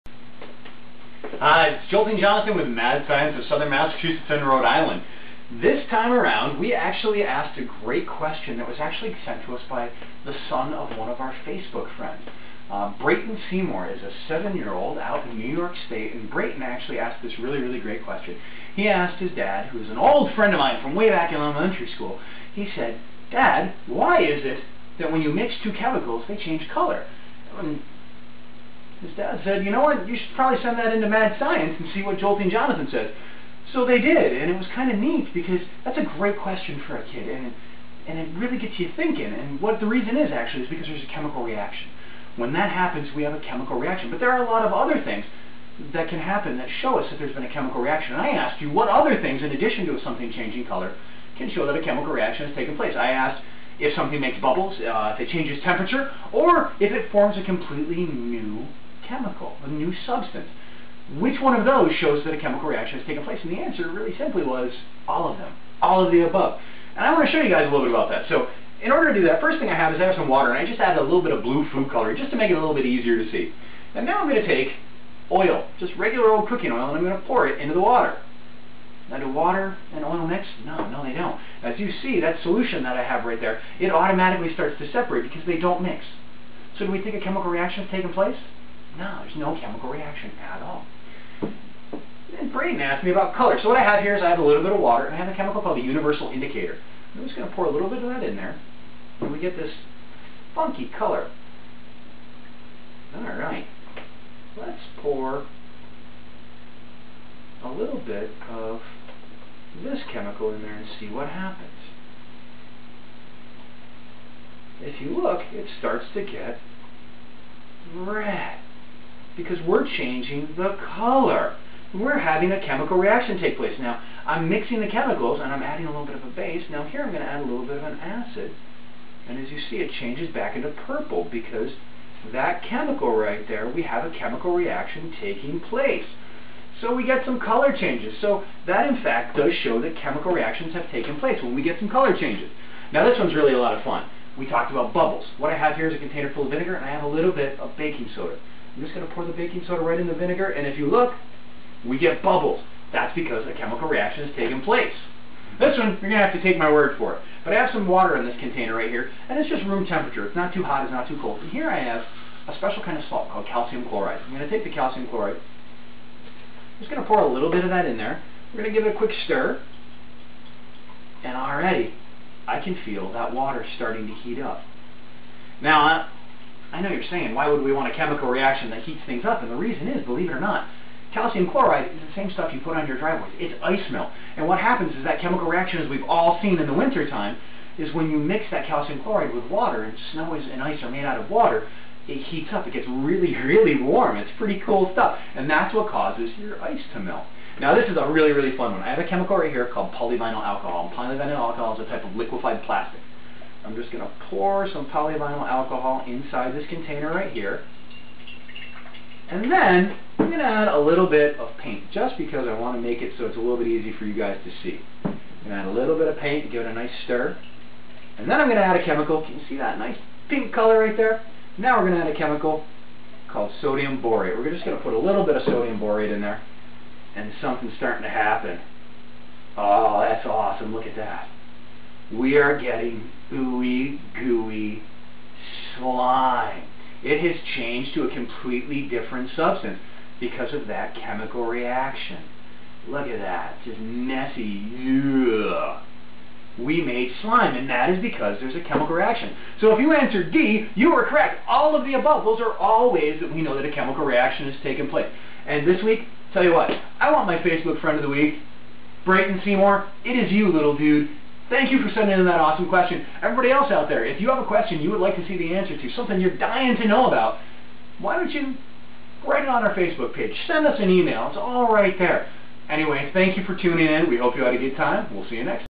Chemical Reactions
Jolting Jonathan of Mad Science of Southern MA & RI answers this week's question.

This week we are looking at a question posed by the son of one of our Facebook fans. Brayton Seymour asked: "Why do chemicals change color when you mix them?" We would expand upon that question and ask in addition to changing color, how can you tell if a chemical reaction has taken place?

A: Change in temperature.
B: Formation of bubbles.
C: Formation of a new substance.